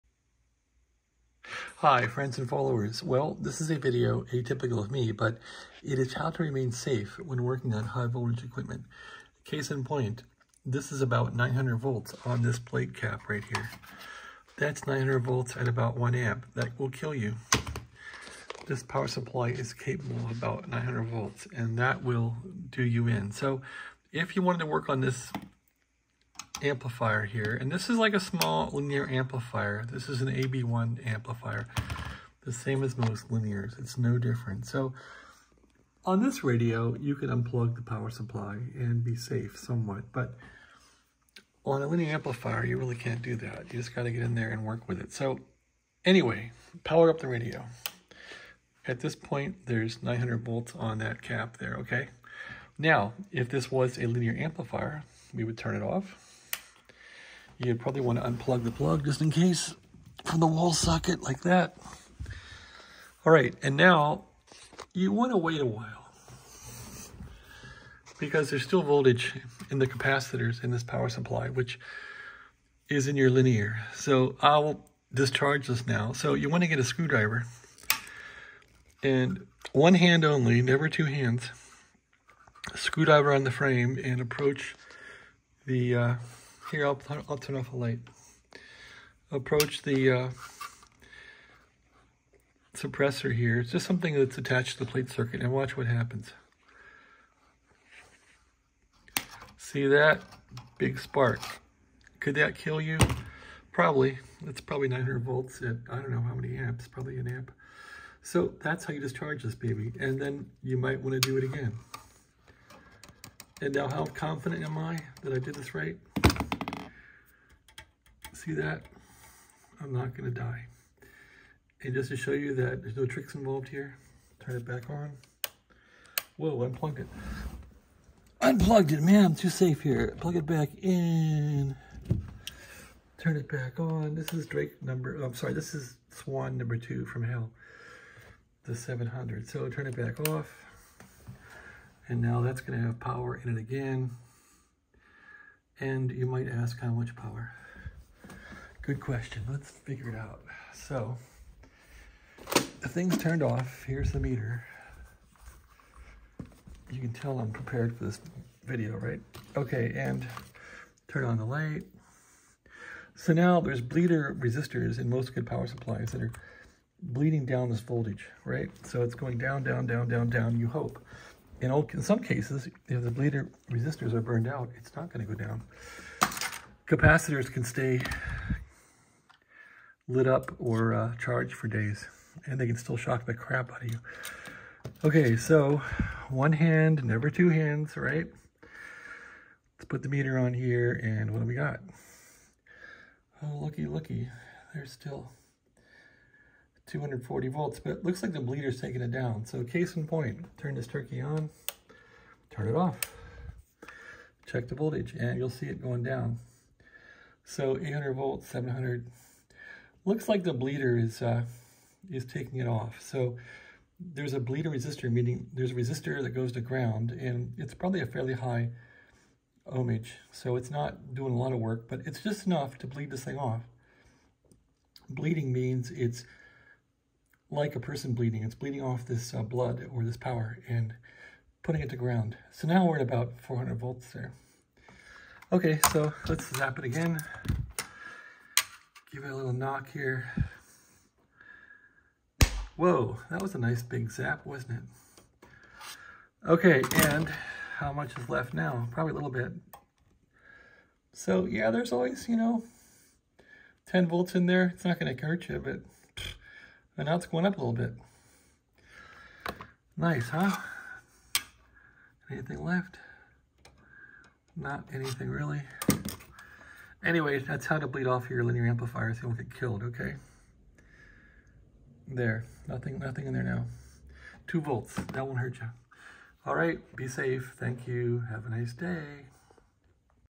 Hi voltage can kill you This is how to safely discharge high-voltage from linear amplifier or finals
Hey Kids.. Electricity can KILL you...

Yeah I borrowed the picture from Mr. BBI